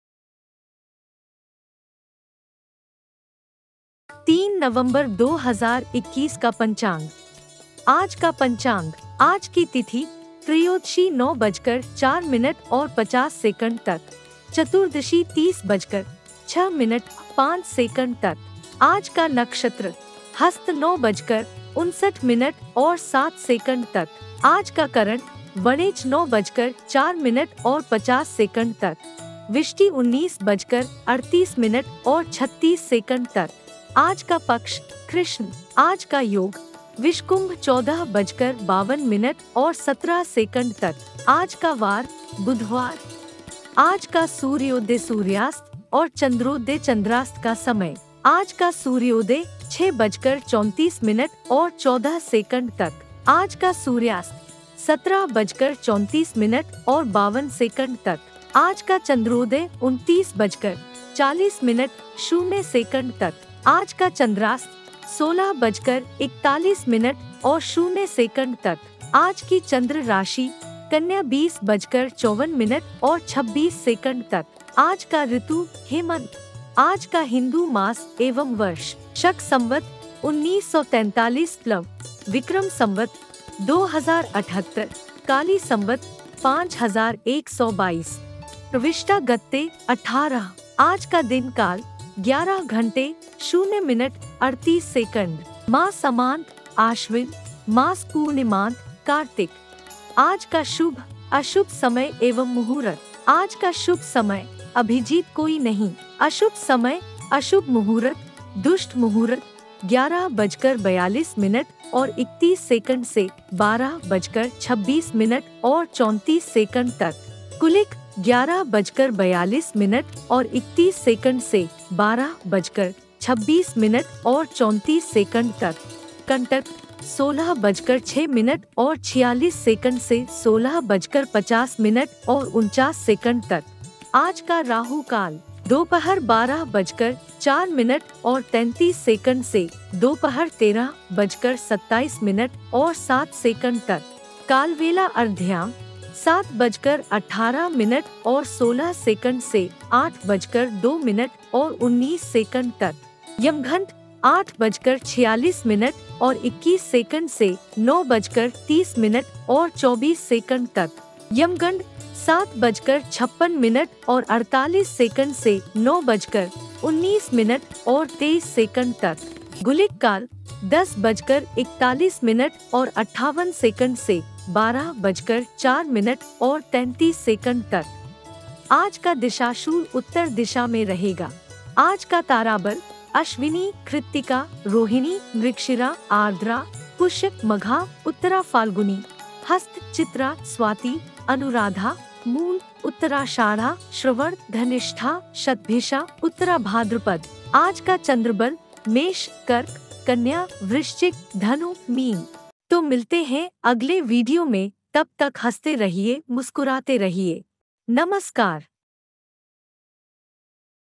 Aaj ka Panchang, 03 November 2021 ka Panchang । Today Hindi Panchang, शुभ मुहूर्त, राहुकाल
Aaj ka Panchang, 01 November 2021 ka Panchang । Wednesday Hindi Panchang, शुभ मुहूर्त, राहुकाल

Auspicious Timings (Shubh Muhurat) & Auspicious Timings (Ashubh Muhurat),